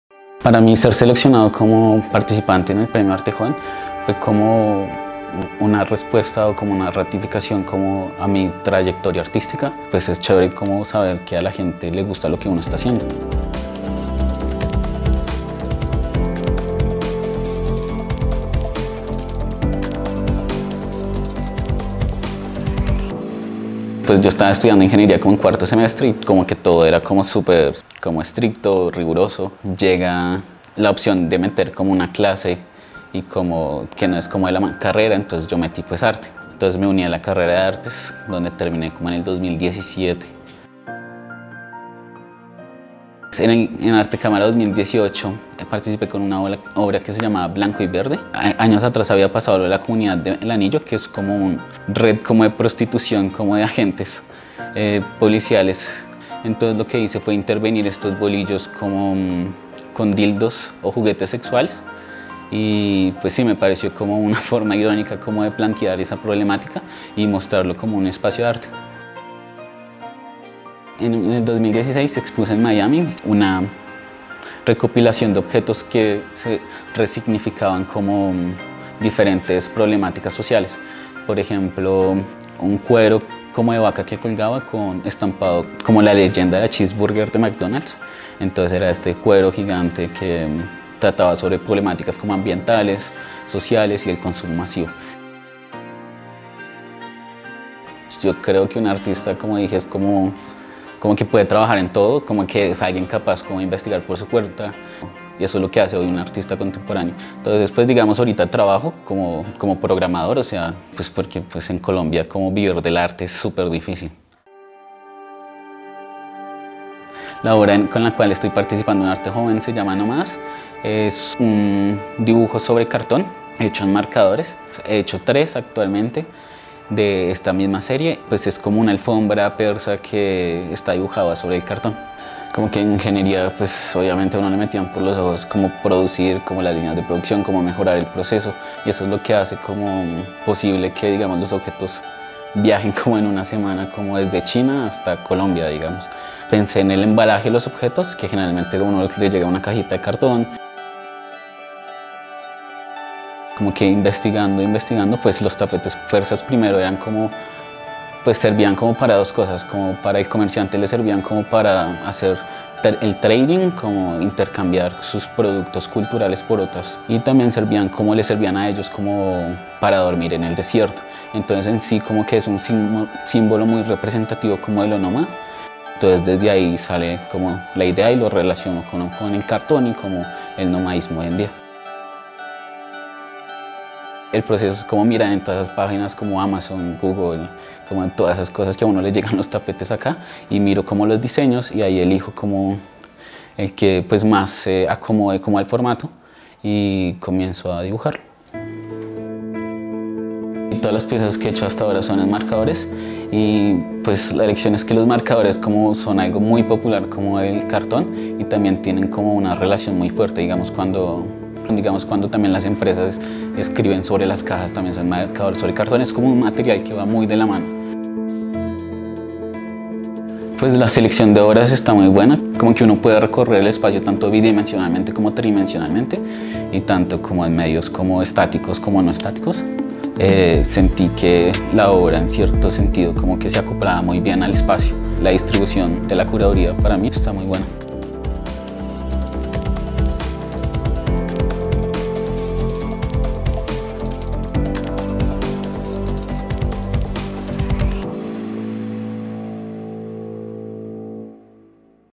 Arte Joven 2021 - Daniel Jiménez
Los finalistas del Premio Arte Joven recibirán cubrimiento especial durante un ciclo de 10 días/10 artistas. El artista del día es Daniel Jiménez y su obra Nómadas.